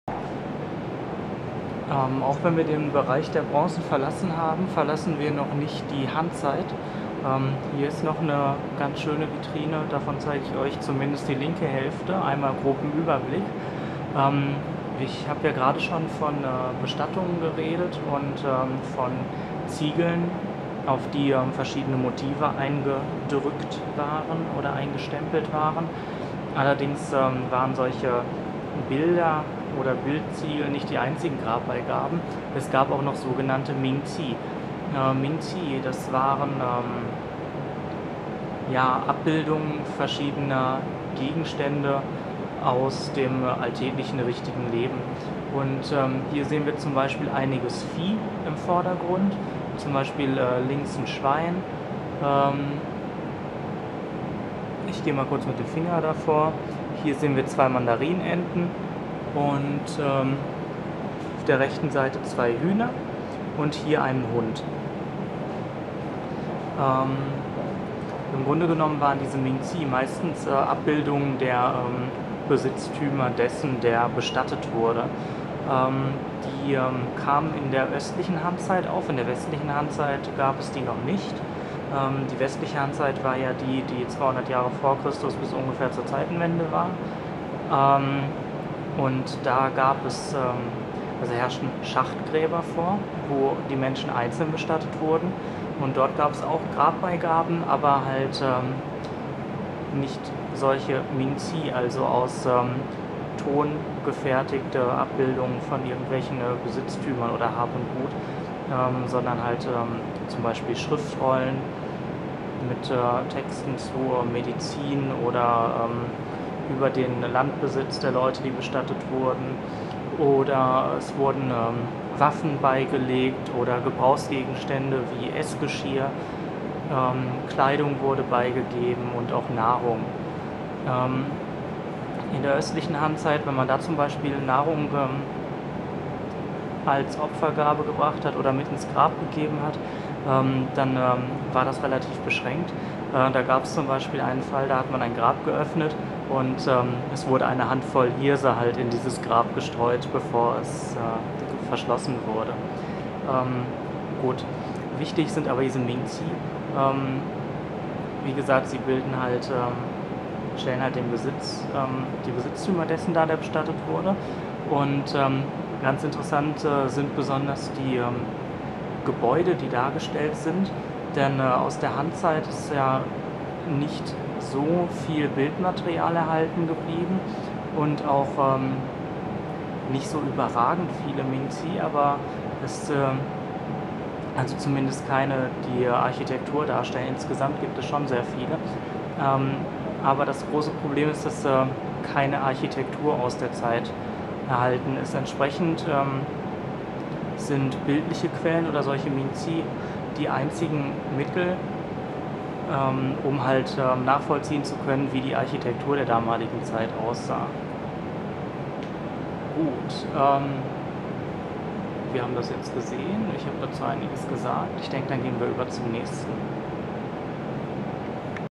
China 2012: Besuch in Ningxia Bowuguan (Teil 4)
Dieses Video zeigt eine Vitrine mit 明器 / 冥器 / 盟器 ming2qi4 aus der Han-Zeit. Der Begriff ming2qi4 bezeichnet Grabbeigaben. Diese änderten sich in Art und Beschaffenheit im Laufe der chinesischen Geschichte mehrfach. Besonders bekannt ist beispielsweise die Terrakotta-Armee des ersten chinesischen Kaiser Qin Shihuang in Xi'an, die realistischen Tonarbeiten mit Darstellungen von Personen, Tieren und Gebrauchsobjekten des täglichen Lebens oder die Dreifarbenkeramik der Tang-Dynastie.
Hier zu sehen sind han-zeitliche Miniaturen verschiedener Nutztiere (Schwein, Enten, Hühner, Hund (v. l. n. r.)) und Architektur. Für die heutige Forschung sind han-zeitliche ming2qi4 von großem Interesse, unter anderem weil sie ein so realistisches Bild von der Alltagskultur bieten. Sie zeigen verschiedenste Architektur, Kleidungen oder Berufe.

Zeit: Han-Dynastie (westliche) 206 v. Chr. - 8. n. Chr.; (östliche) 25. n. Chr. - 22. n. Chr.